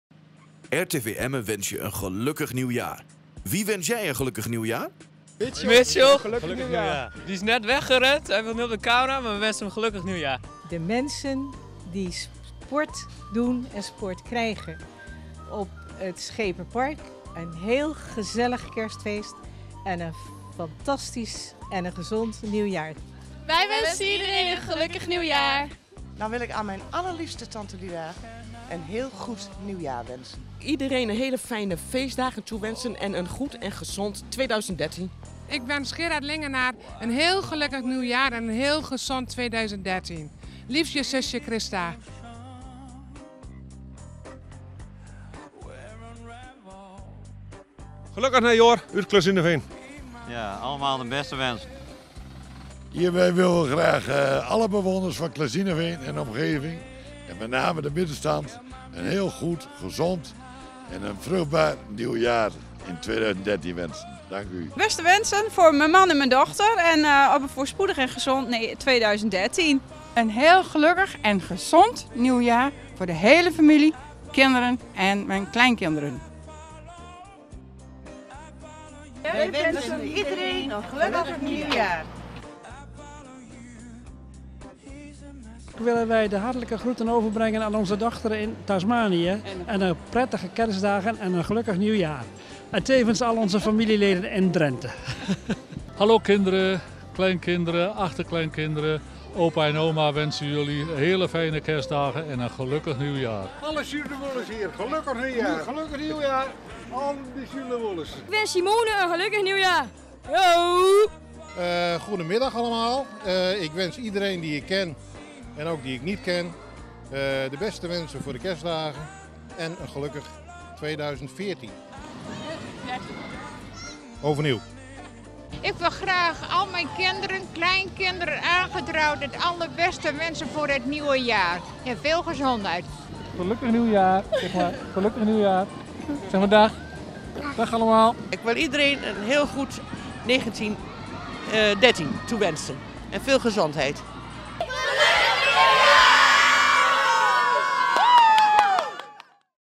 RTV Emmen - Nieuwjaarsgroet 2013
De laatste dagen van 2012 zijn aangebroken. Wij reden een rondje door Zuidoost Drenthe om iedereen de kans te geven zijn nieuwjaarsgroet op te nemen.

Ook RTV Emmen wenst iedereen alvast een gelukkig en gezond nieuw jaar toe!